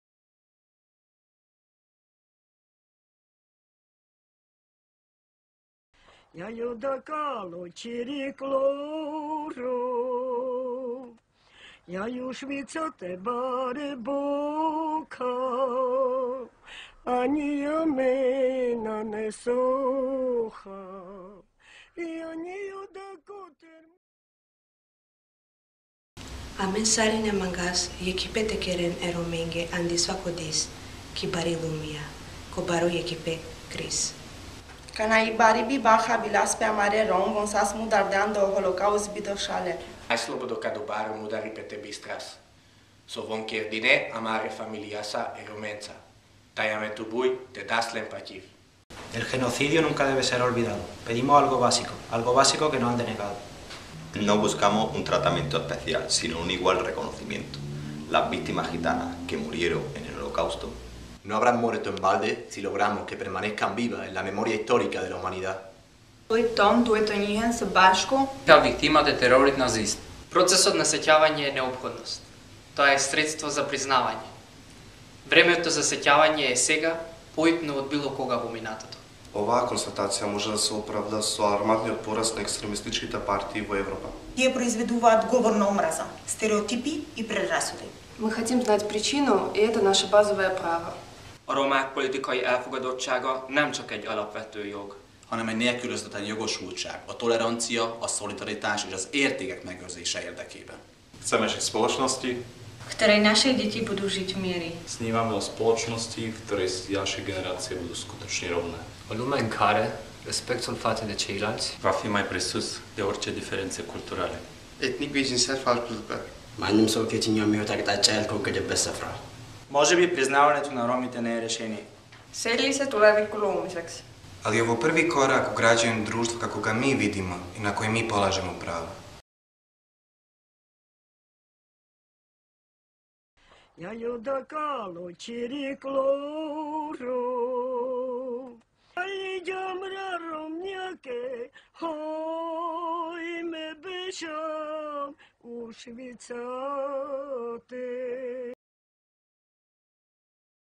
Students' reaction against the discrimination of the Roma Holocaust victims
Students from the Central European University, Budapest are asking for an official recognition and representation of the Roma Holocaust victims in the official Holocaust ceremonies.